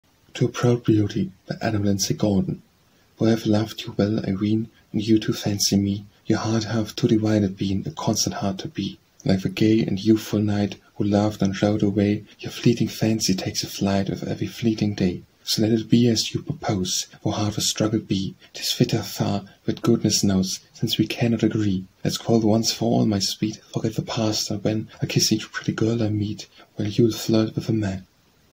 To a Proud Beauty - Adam Lindsay Gordon
To A Proud Beauty

    By Adam Lindsay Gordon

    Though I have loved you well, I ween,
    And you, too, fancied me,
    Your heart hath too divided been
    A constant heart to be.
    And like the gay and youthful knight,
    Who loved and rode away,
    Your fleeting fancy takes a flight
    With every fleeting day.

    So let it be as you propose,
    Tho hard the struggle be;
    Tis fitter far, that goodness knows!
    Since we cannot agree.
    Let s quarrel once for all, my sweet,
    Forget the past, and then
    I ll kiss each pretty girl I meet,
    While you ll flirt with the men.